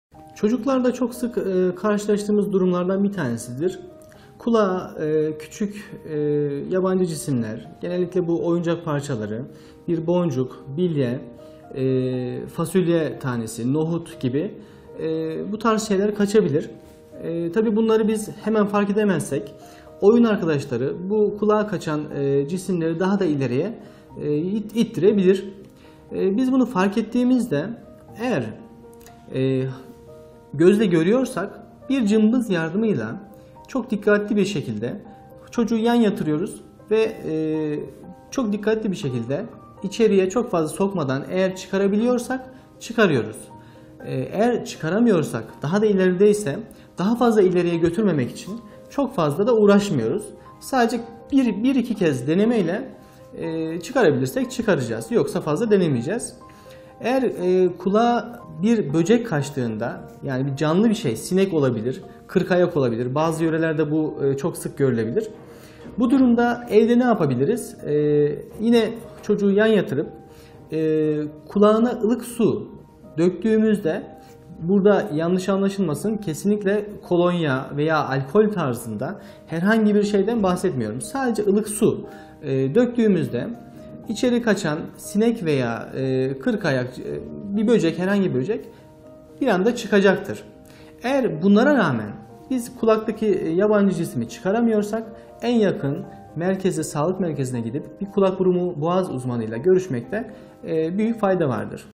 Çocuğumun Kulağına Bir Şey Kaçtı Ne Yapmalıyım?
Çocuğumun Kulağına Bir Şey Kaçtı Ne Yapmalıyım? Dr. Şakir AYDOĞDU sizin için yanıtlıyor.

38001716